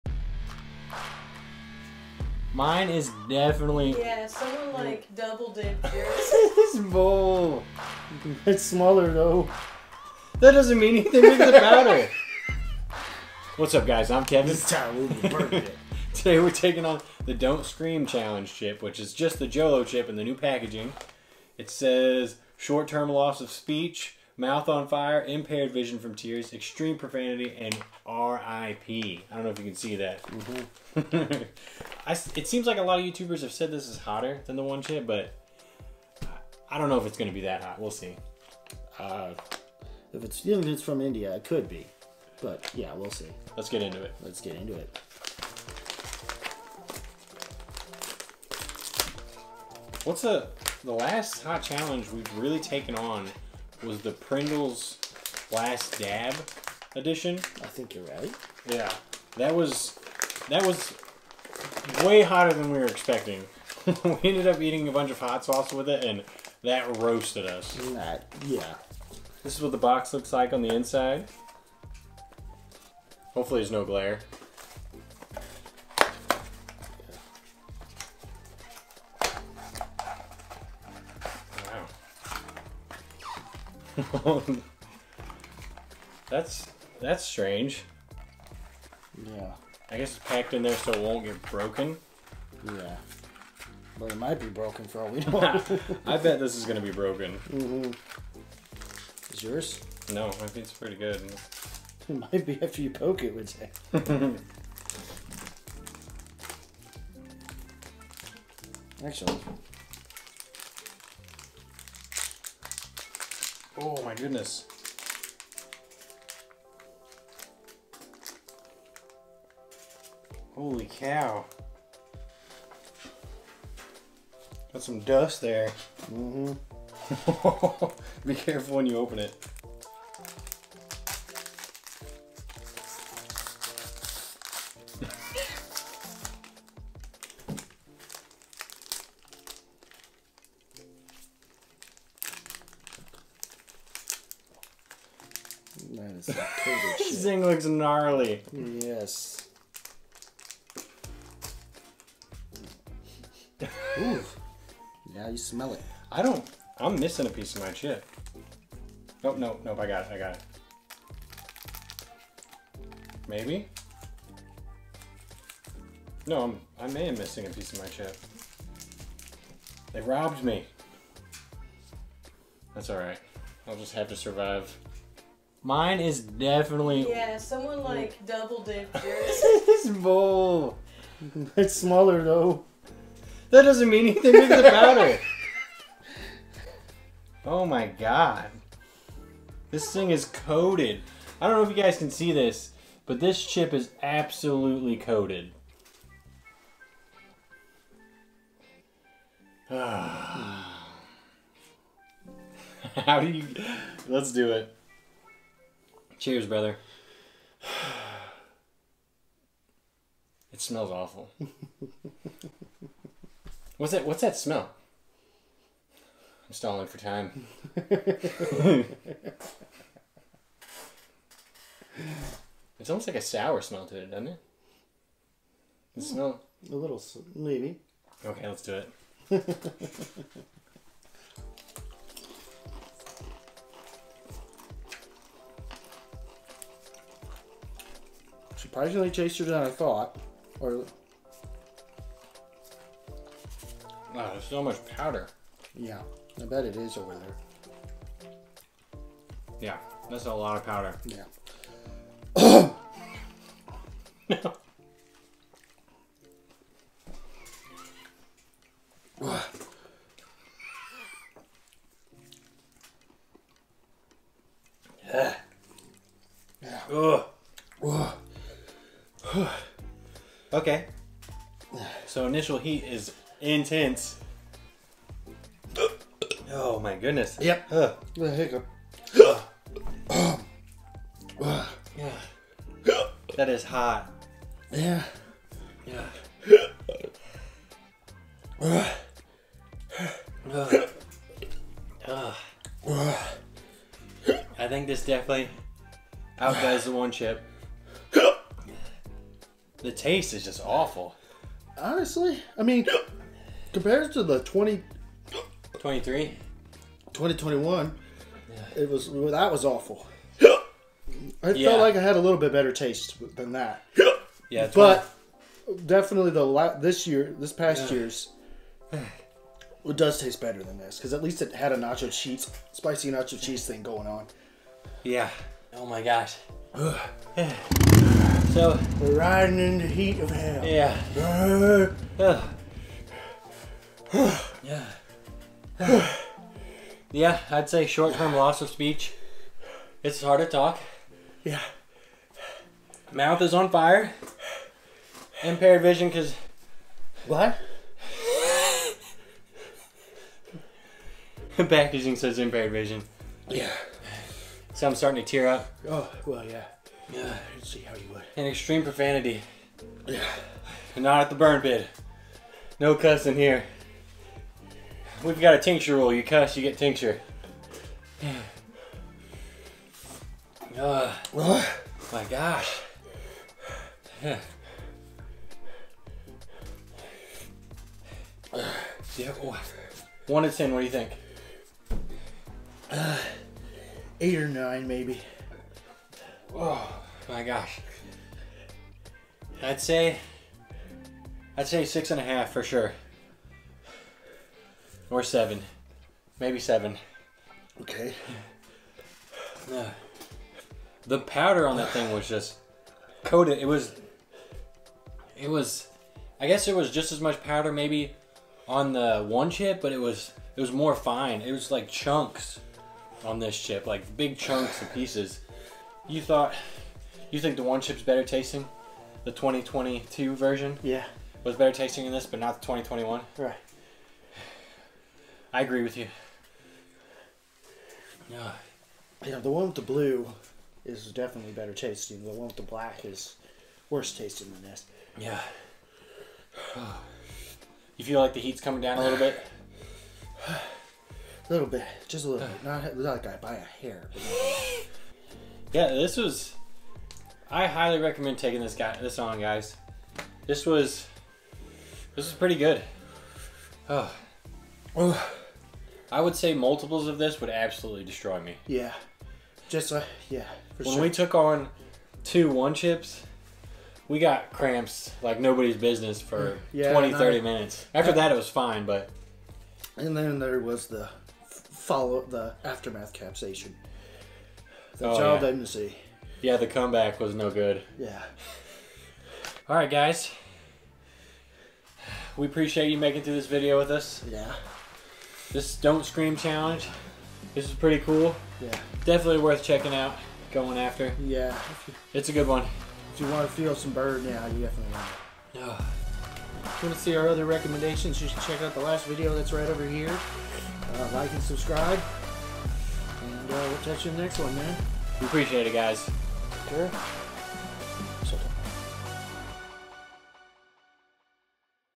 Jolo Chip (DON'T SCREAM EDITION)
Watch us take on the JOLO CHIP (NEW EDITION)- LAST CHIP CHALLENGE! JOLOCHIP is packed with heat from The Carolina Reaper pepper, Ghost pepper and Trinidad Scorpion. Did it burn hotter than the One Chip Challenge for Kevin? Will it get the hiccup seal of approval from Tyler? Join us as we burn!